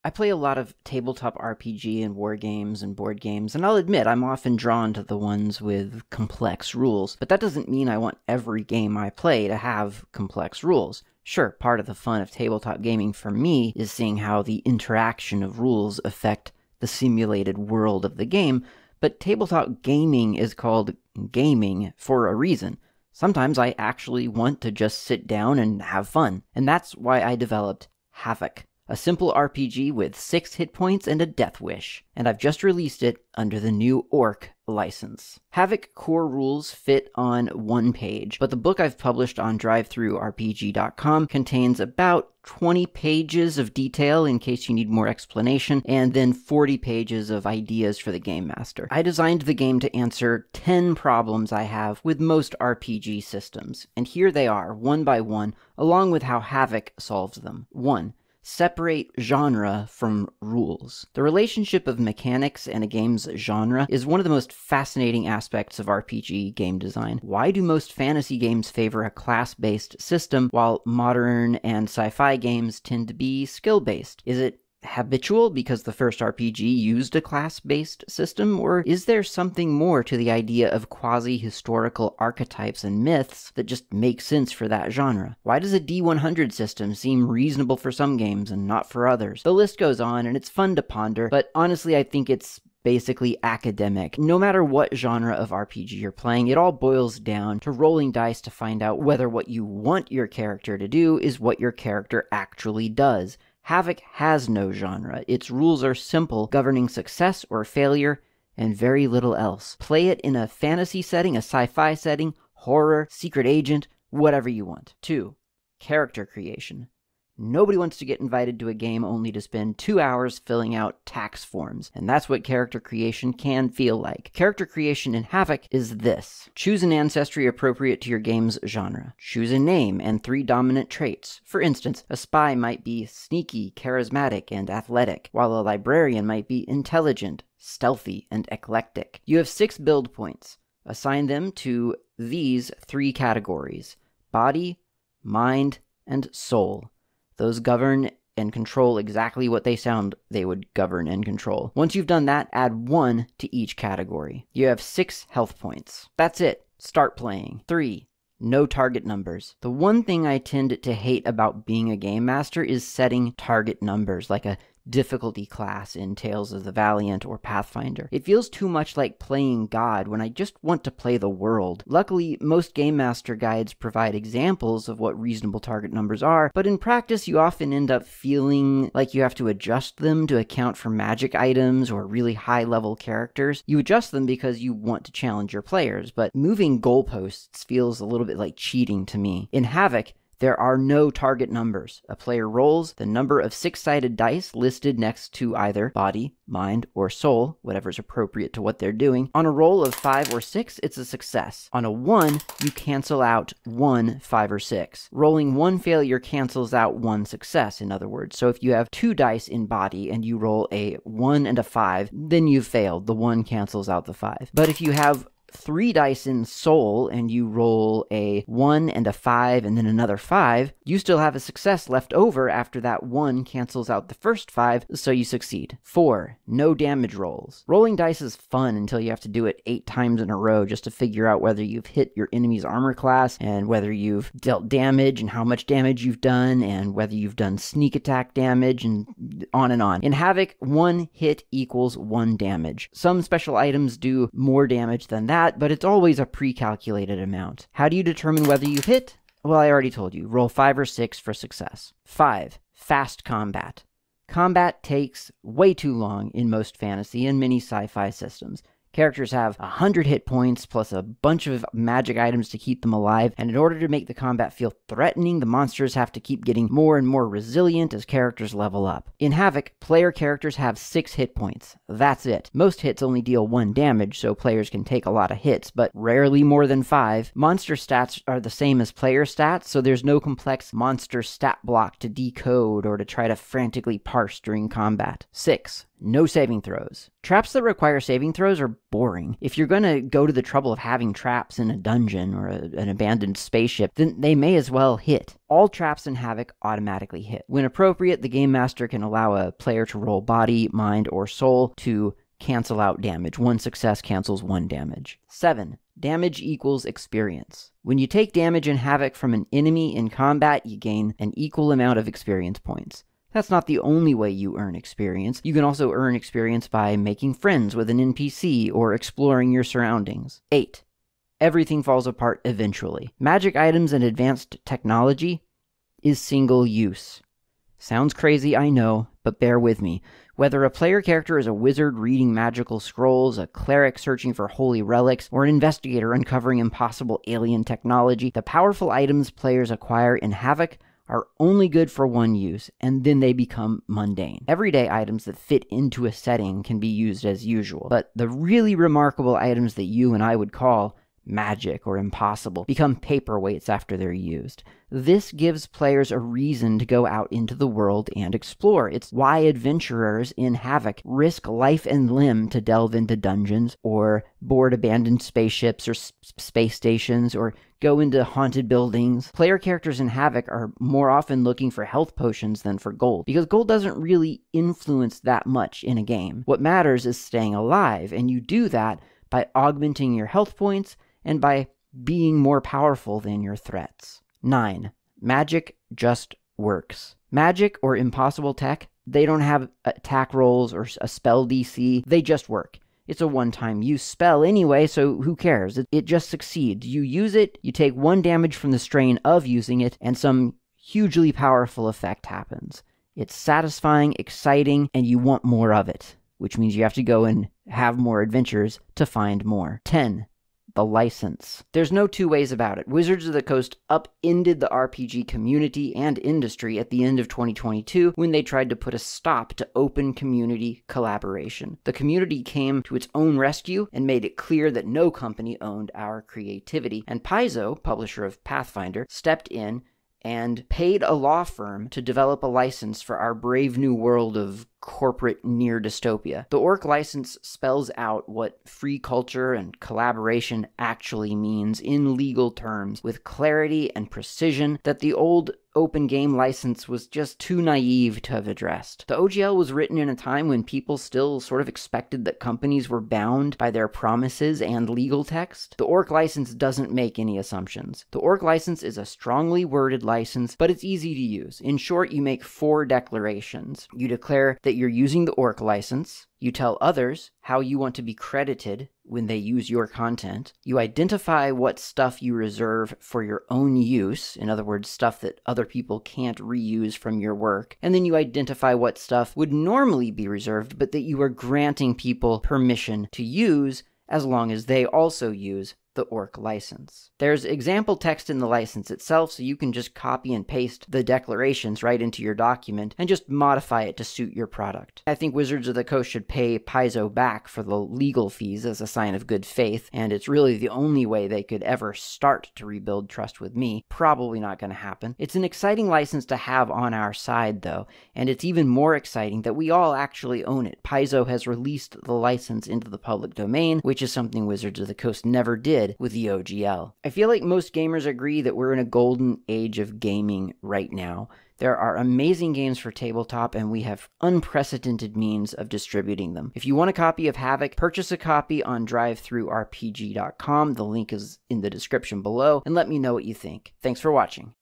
Why I'm using the ORC license for my new RPG
There are lots of great reasons to use the orc license by paizo, and now that it's got a final draft I'm using it for my new game.